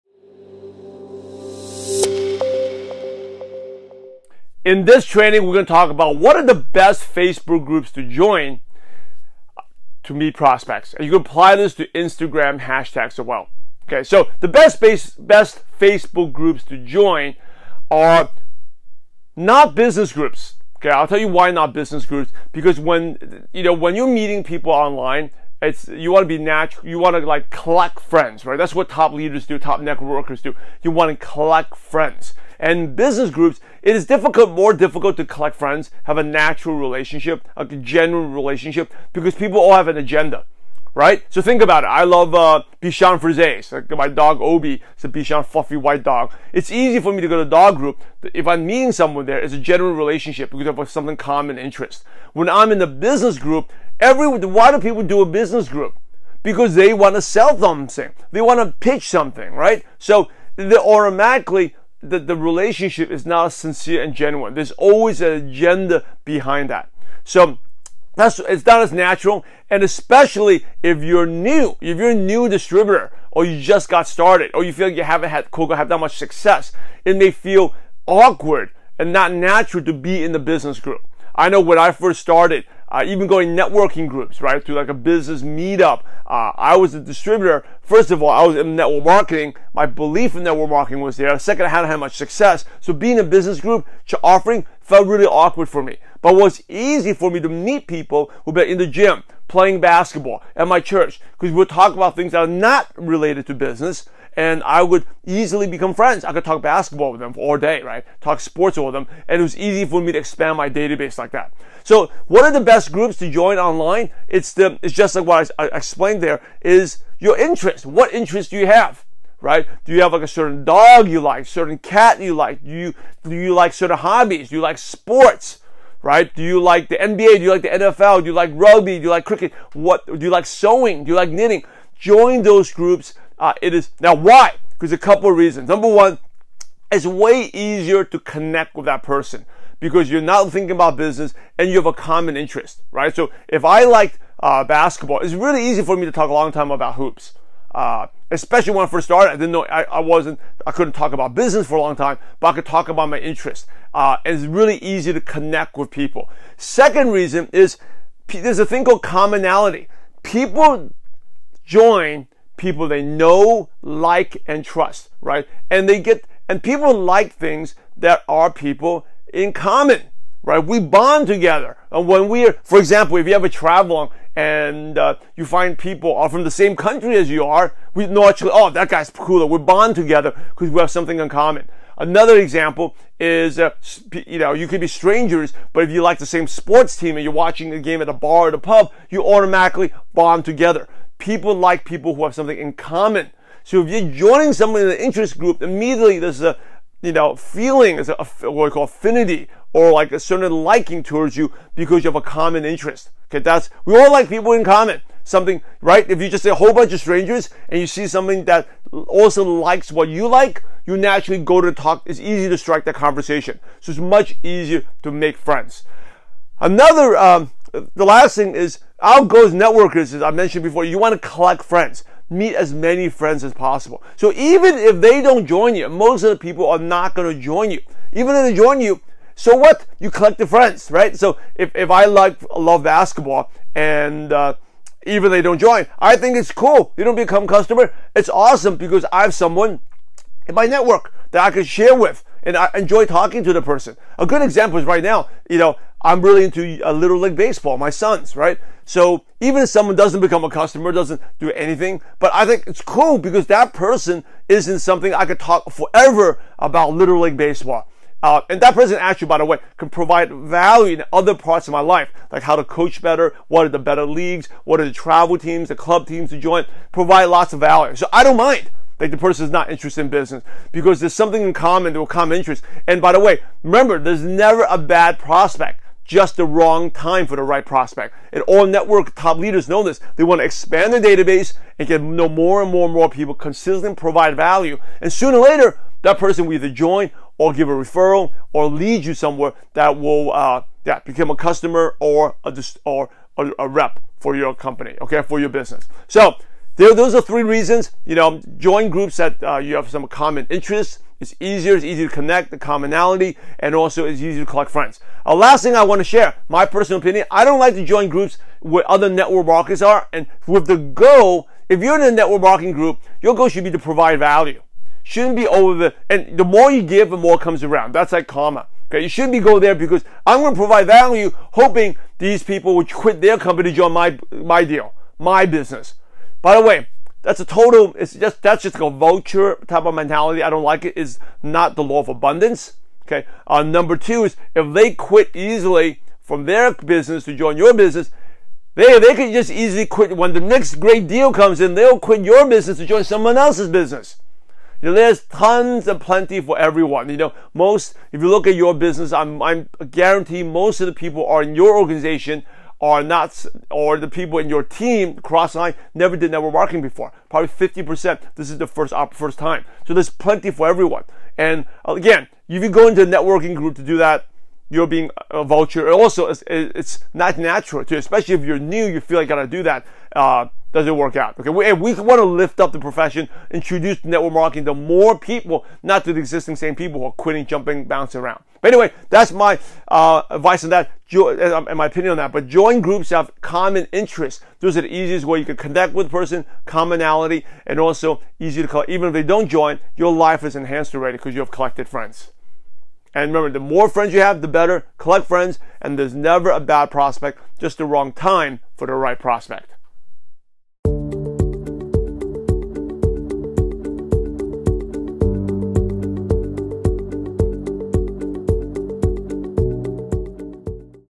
The Best Facebook Groups for Network Marketing Prospecting
How to Find Prospects Using Facebook Groups

How do you find good prospects on Facebook?

👉🏼 Watch the video and learn how to use Facebook Groups and the best ones to join.